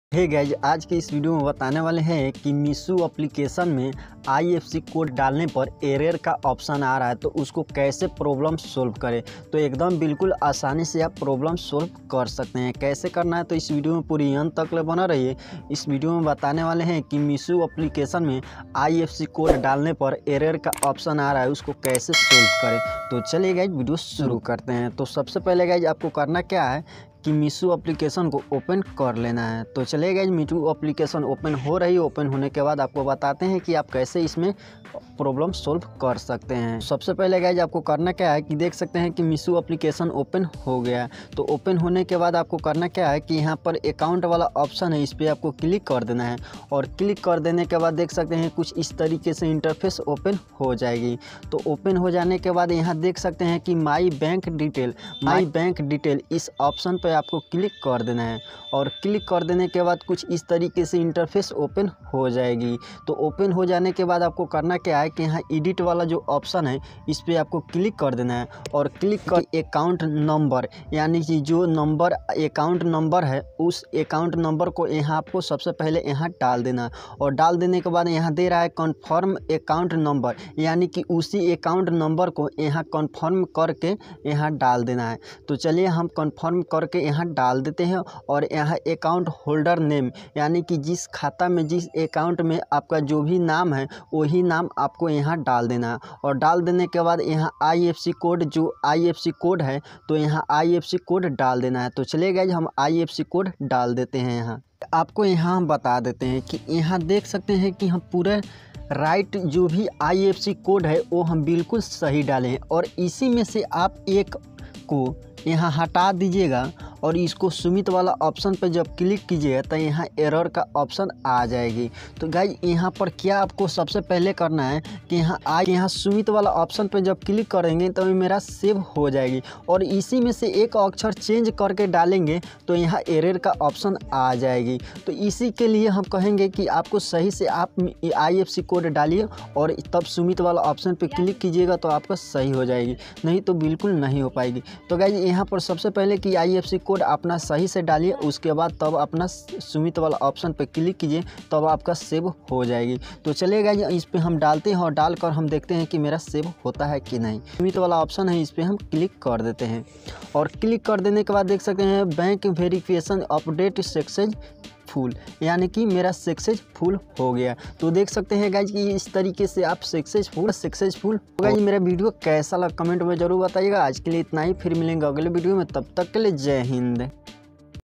Meesho Me Ifsc Code Problem Solve || Meesho Me Ifsc Code Kaise Pata Kare
Meesho Me Ifsc Code Problem Solve || Meesho Me Ifsc Code Kaise Pata Kare || Dosto Is Video Me Maine Bataya Hai Ki Meesho Me Ifsc Code Problem Solve Dosto Video Pasand Aaye to Channel ko Subscribe Karna Na Bhule Aur Share Jarur Kare